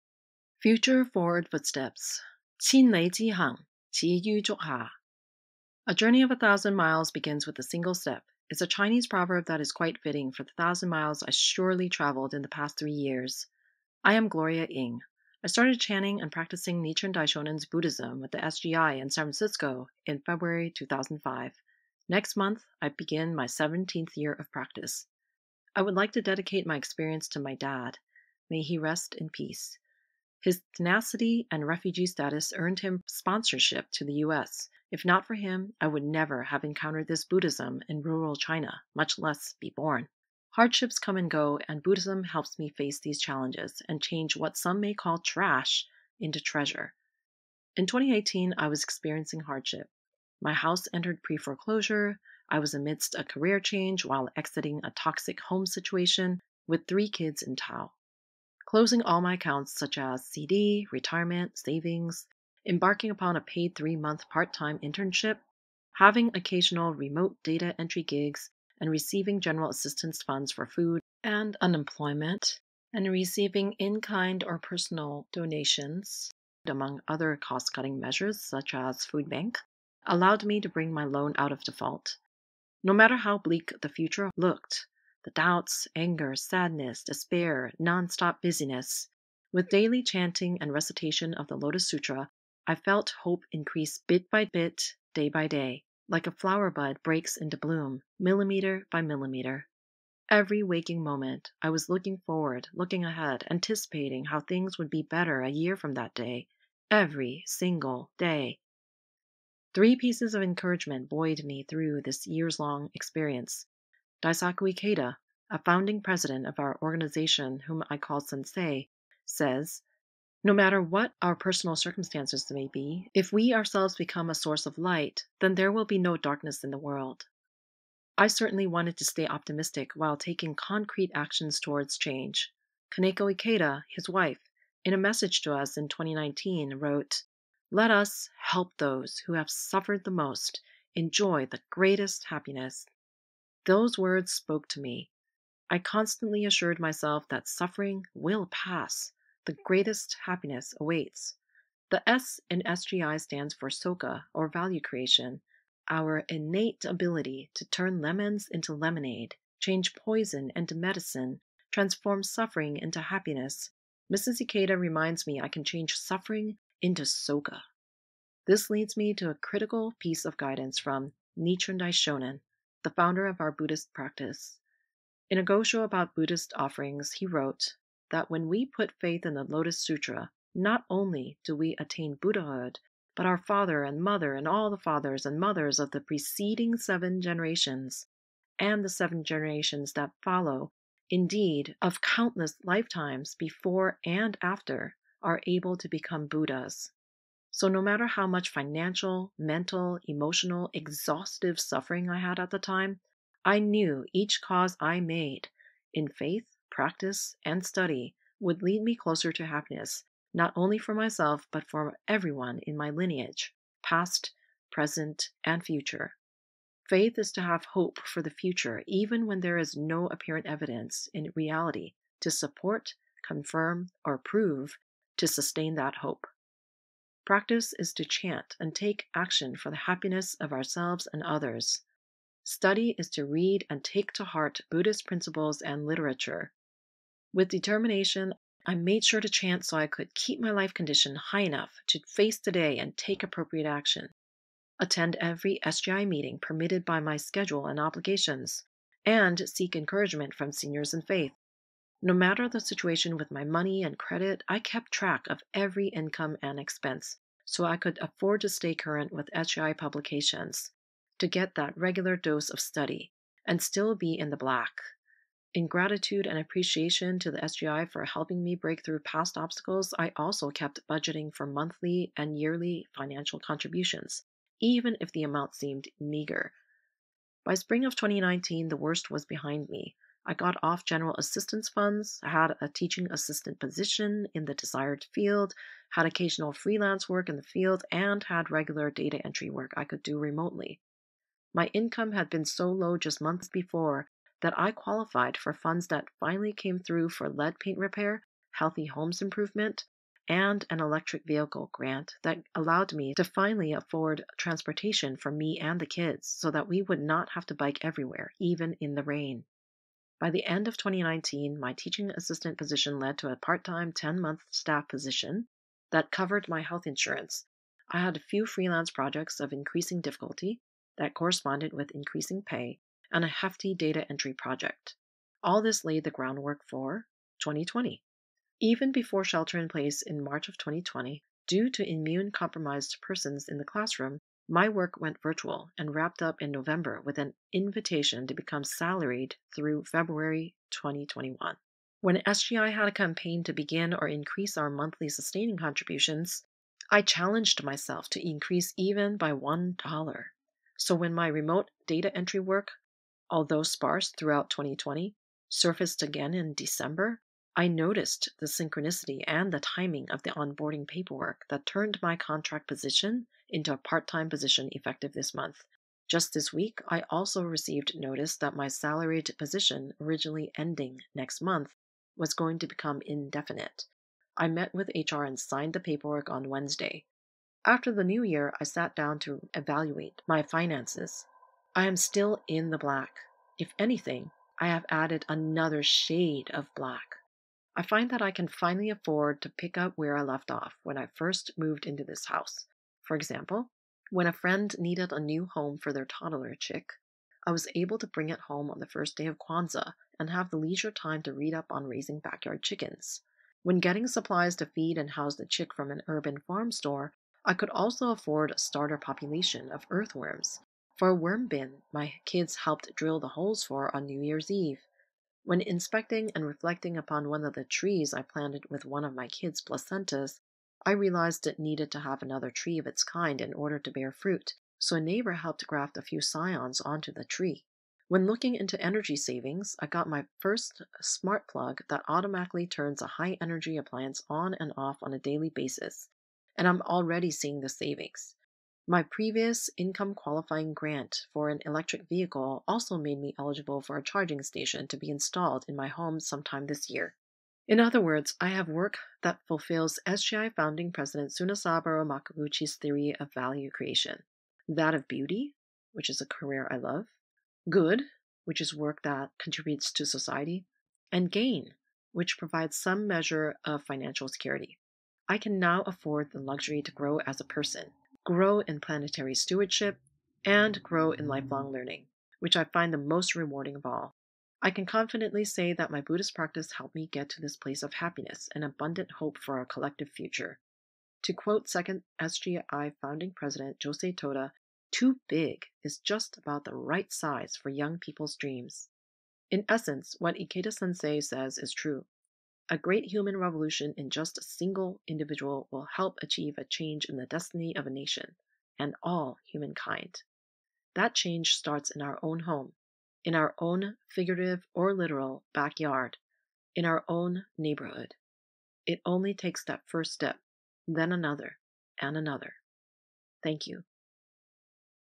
Future-Forward Footsteps by Gloria Ng, 16-Jan-21, dedicated to Hon Keung Ng
SGI-USA Nichiren Buddhist practitioner shares about overcoming challenges in her immediate environment.